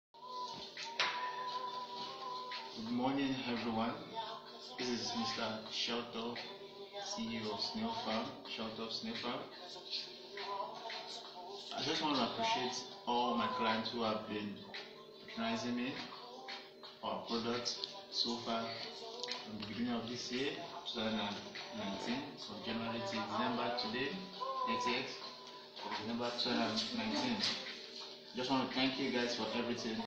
MR SHELLTOP SENDING MESSAGE OF APPRECIATION TO ALL HIS CLIENTS ND FANS
Snail Farming in Bayelsa state